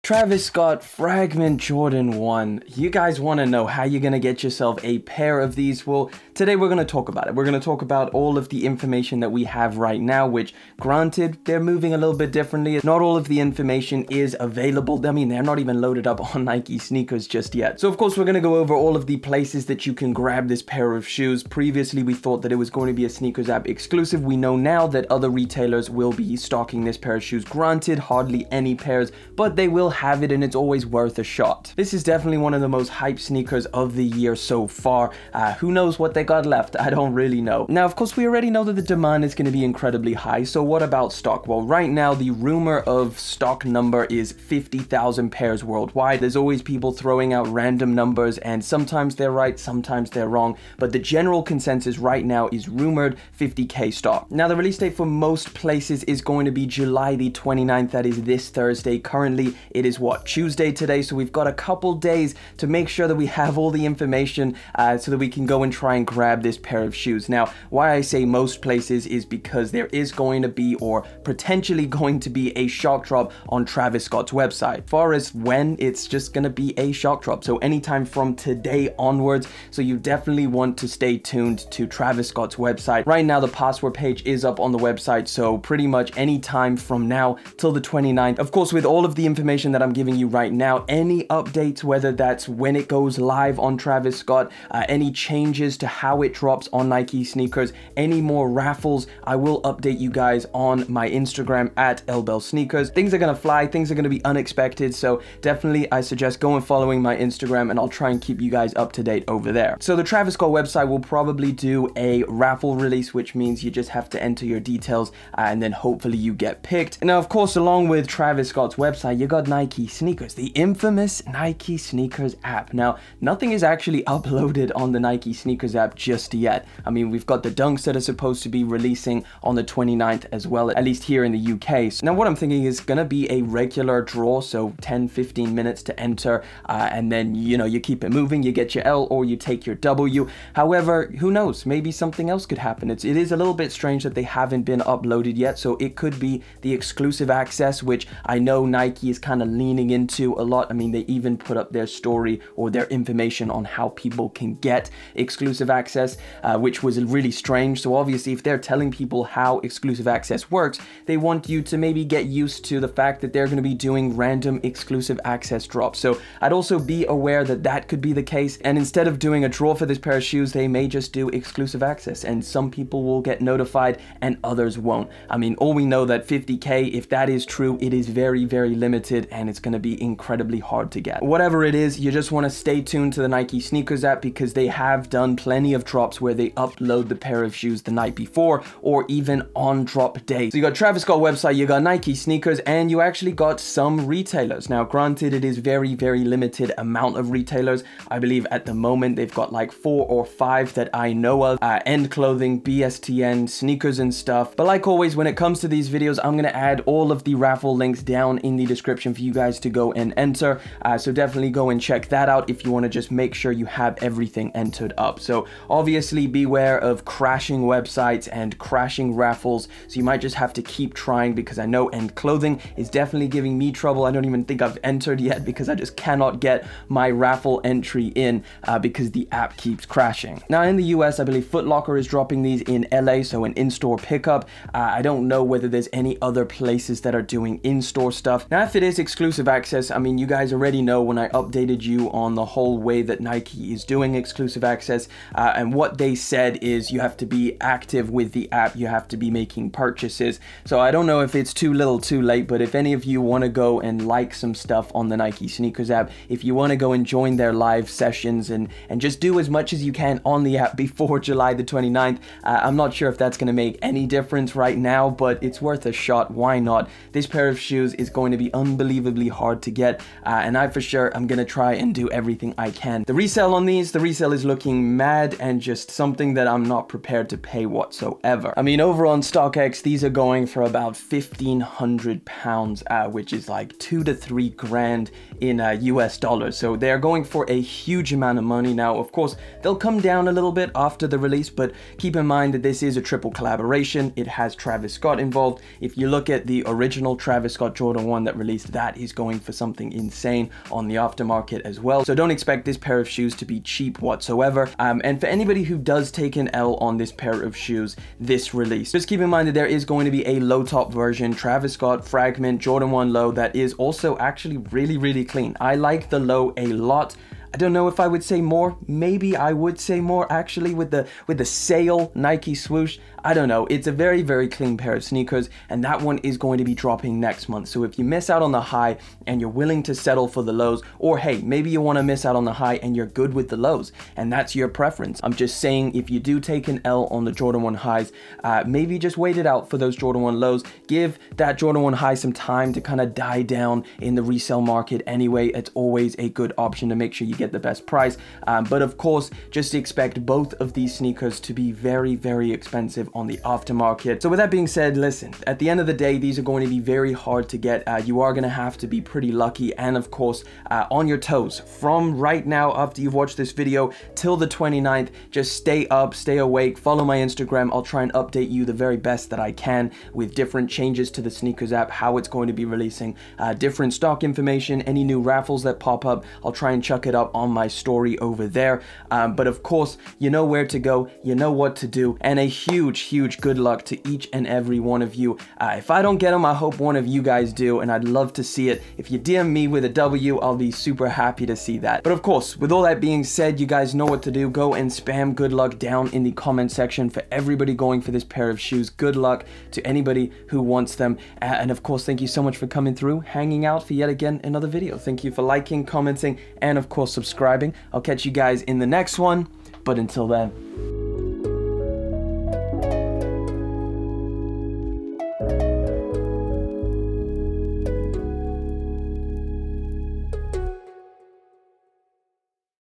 BETTER CHANCE AT GETTING THESE NOW!? How To Cop The Jordan 1 Travis Scott X Fragment
. Raffles To Enter:
EU

Nike SNKRS App
App Raffle

US
Nike SNKRS App
App Raffle

Possible Drops

Footlocker US